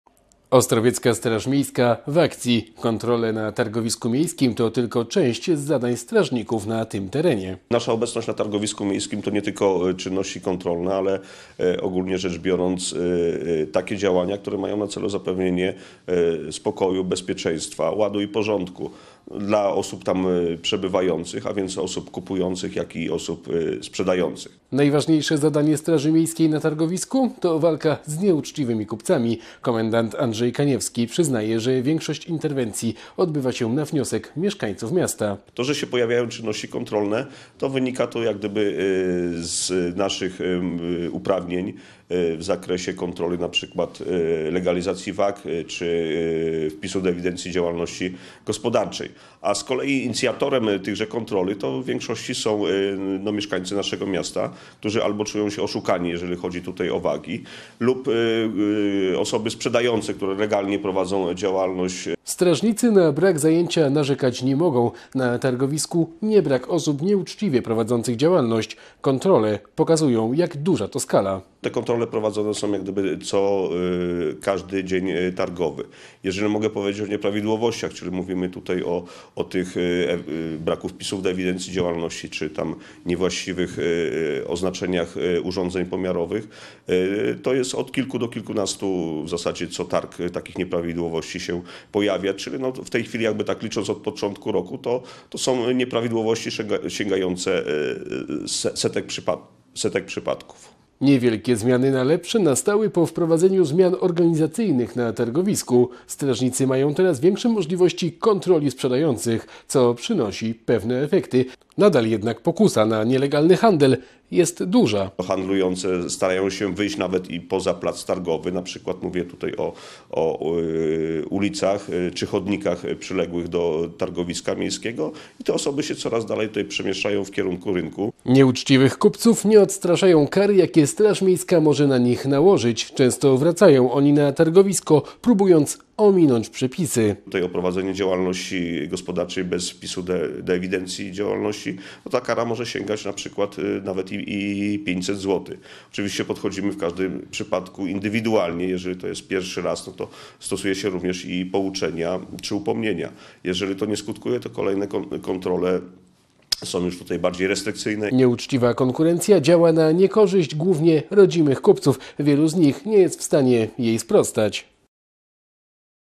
Nieuchwytny cel (ostrowiecka Straż Miejska walczy z szarą strefą)
W każdy dzień targowy ostrowiecka straż miejska walczy z nieuczciwymi kupcami na targowisku miejskim. Okazuje się, że chętnych, aby powiększać szarą strefę, w dalszym ciągu nie brakuje. Nieuczciwa konkurencja odbija się szczególnie na polskich kupcach.